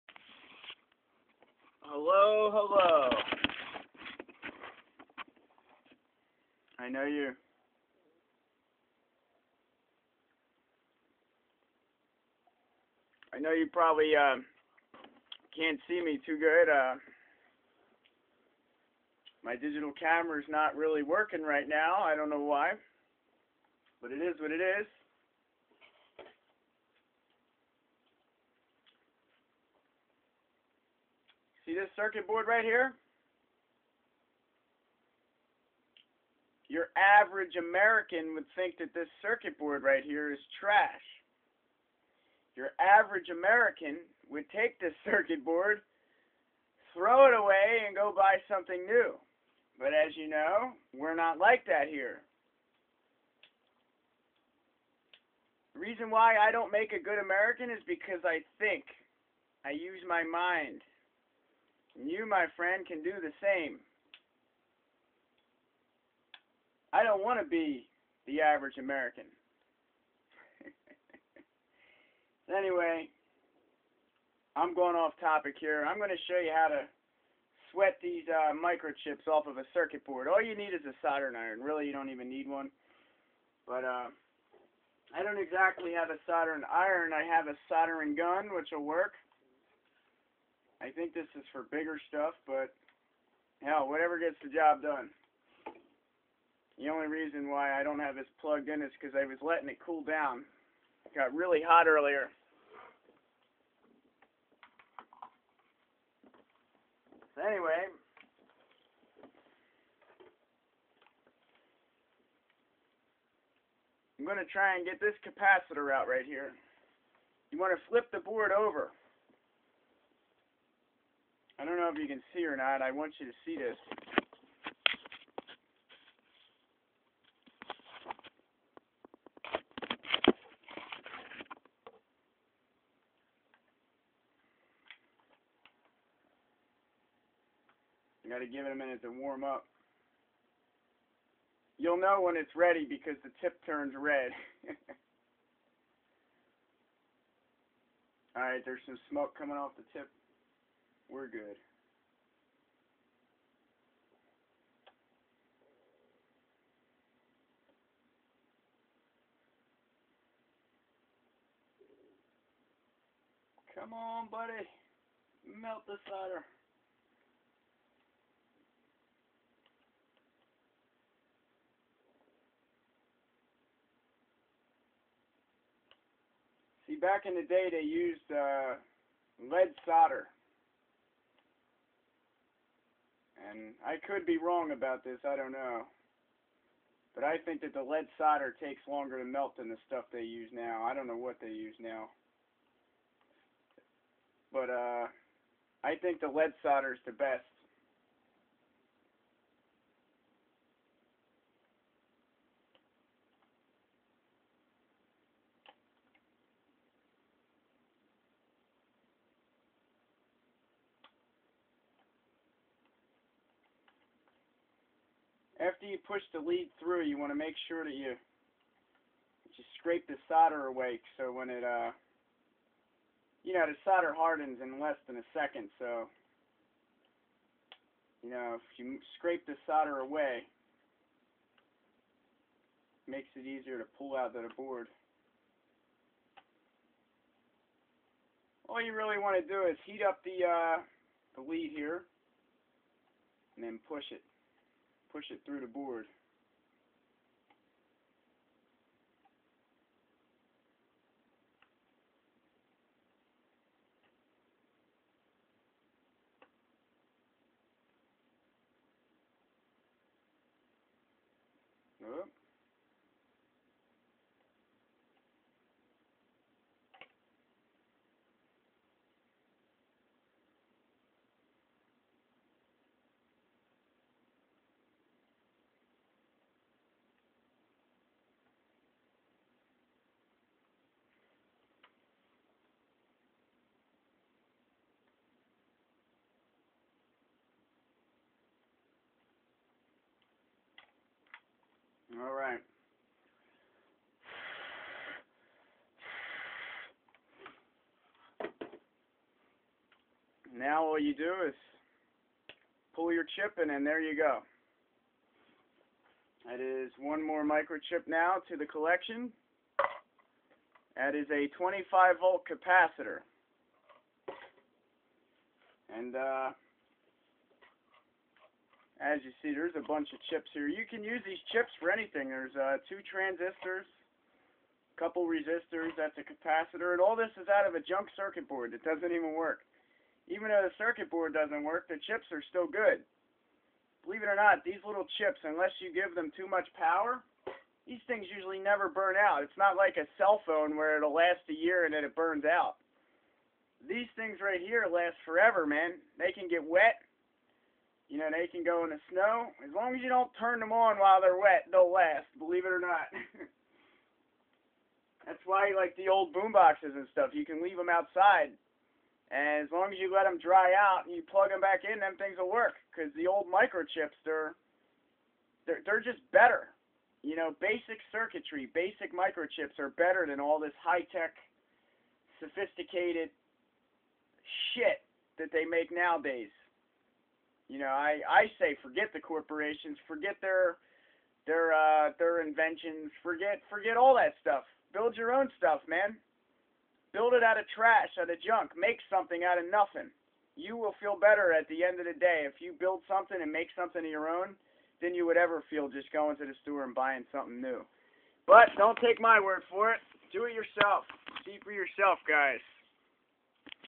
SALVAGING AN OLD CAPACITOR FROM CIRCUIT BOARD
Just me saving a capacitor from an old radio that doesent play anymore. im probabaly going to build some new kind of circuit with it